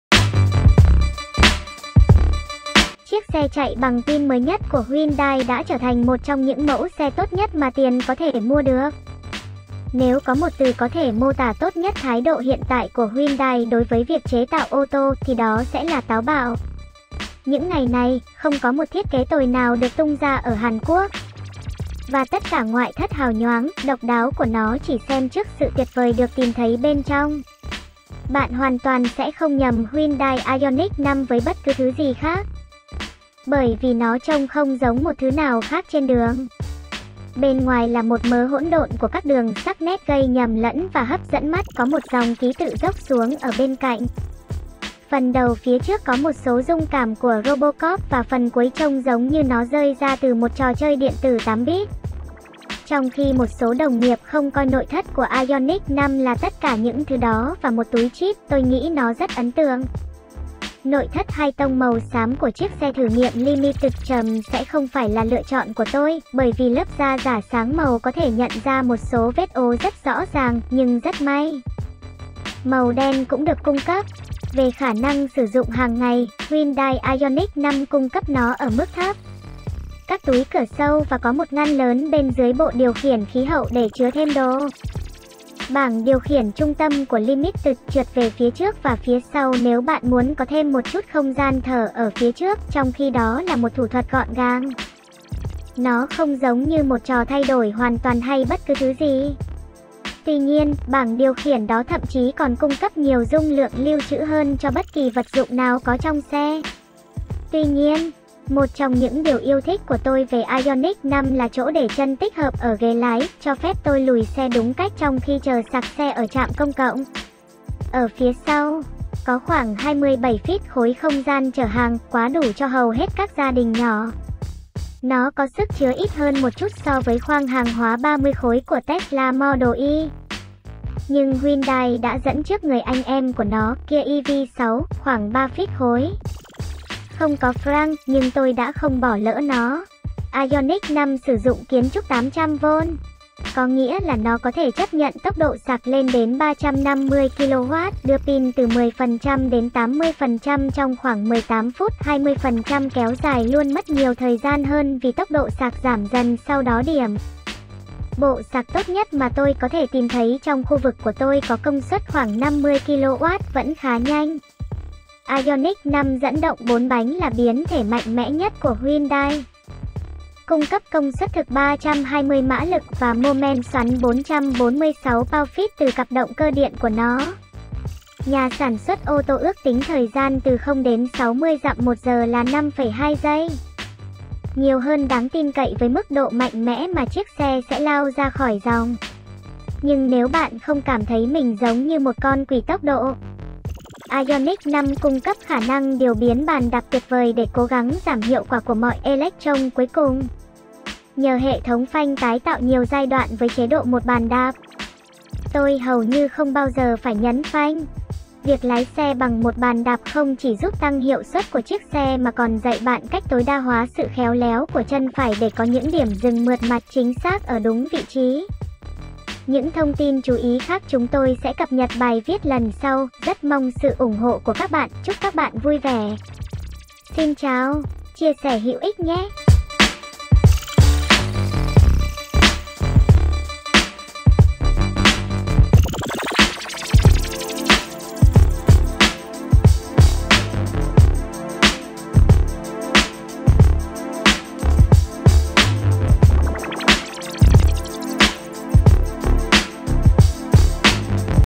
Đánh giá Hyundai Ioniq 5 2022 Ấn tượng Hella
Chiếc xe chạy bằng pin mới nhất của Hyundai đã trở thành một trong những mẫu xe tốt nhất mà tiền có thể mua được.

Nếu có một từ có thể mô tả tốt nhất thái độ hiện tại của Hyundai đối với việc chế tạo ô tô, thì đó sẽ là "táo bạo". Những ngày này, không có một thiết kế tồi nào được tung ra ở Hàn Quốc, và tất cả ngoại thất hào nhoáng, độc đáo của nó chỉ xem trước sự tuyệt vời được tìm thấy bên trong.

Bạn hoàn toàn sẽ không nhầm Hyundai Ioniq 5 với bất cứ thứ gì khác, bởi vì nó trông không giống một thứ nào khác trên đường. Bên ngoài là một mớ hỗn độn của các đường sắc nét gây nhầm lẫn và hấp dẫn mắt; có một dòng ký tự dốc xuống ở bên cạnh, phần đầu phía trước có một số rung cảm của Robocop và phần cuối trông giống như nó rơi ra từ một trò chơi điện tử 8-bit.

Trong khi một số đồng nghiệp không coi nội thất của Ioniq 5 là tất cả những thứ đó và một túi chip, tôi nghĩ nó rất ấn tượng. Nội thất hai tông màu xám của chiếc xe thử nghiệm Limited-trim sẽ không phải là lựa chọn của tôi, bởi vì lớp da giả sáng màu có thể nhận ra một số vết ố rất rõ ràng, nhưng rất may, màu đen cũng được cung cấp.

Về khả năng sử dụng hàng ngày, Hyundai Ioniq 5 cung cấp nó ở mức thấp. Các túi cửa sâu và có một ngăn lớn bên dưới bộ điều khiển khí hậu để chứa thêm đồ. Bảng điều khiển trung tâm của Limited trượt về phía trước và phía sau nếu bạn muốn có thêm một chút không gian thở ở phía trước; trong khi đó là một thủ thuật gọn gàng, nó không giống như một trò thay đổi hoàn toàn hay bất cứ thứ gì. Tuy nhiên, bảng điều khiển đó thậm chí còn cung cấp nhiều dung lượng lưu trữ hơn cho bất kỳ vật dụng nào có trong xe. Tuy nhiên, một trong những điều yêu thích của tôi về Ioniq 5 là chỗ để chân tích hợp ở ghế lái, cho phép tôi lùi xe đúng cách trong khi chờ sạc xe ở trạm công cộng.

Ở phía sau, có khoảng 27 feet khối không gian chở hàng, quá đủ cho hầu hết các gia đình nhỏ. Nó có sức chứa ít hơn một chút so với khoang hàng hóa 30 khối của Tesla Model Y, nhưng Hyundai đã dẫn trước người anh em của nó, Kia EV6, khoảng 3 feet khối. Không có frunk, nhưng tôi đã không bỏ lỡ nó.

Ioniq 5 sử dụng kiến ​​trúc 800 volt, có nghĩa là nó có thể chấp nhận tốc độ sạc lên đến 350 kilowatt, đưa pin từ 10% đến 80% trong khoảng 18 phút (20% kéo dài luôn mất nhiều thời gian hơn vì tốc độ sạc giảm dần sau đó điểm). Bộ sạc tốt nhất mà tôi có thể tìm thấy trong khu vực của tôi có công suất khoảng 50 kW, vẫn khá nhanh.

Ioniq 5 dẫn động bốn bánh là biến thể mạnh mẽ nhất của Hyundai, cung cấp công suất thực 320 mã lực và mô-men xoắn 446 pound-feet từ cặp động cơ điện của nó. Nhà sản xuất ô tô ước tính thời gian từ 0 đến 60 dặm / giờ là 5,2 giây, nhiều hơn đáng tin cậy với mức độ mạnh mẽ mà chiếc xe sẽ lao ra khỏi dòng. Nhưng nếu bạn không cảm thấy mình giống như một con quỷ tốc độ, Ioniq 5 cung cấp khả năng điều biến bàn đạp tuyệt vời để cố gắng giảm hiệu quả của mọi electron cuối cùng.

Nhờ hệ thống phanh tái tạo nhiều giai đoạn với chế độ một bàn đạp, tôi hầu như không bao giờ phải nhấn phanh. Việc lái xe bằng một bàn đạp không chỉ giúp tăng hiệu suất của chiếc xe mà còn dạy bạn cách tối đa hóa sự khéo léo của chân phải để có những điểm dừng mượt mà, chính xác ở đúng vị trí.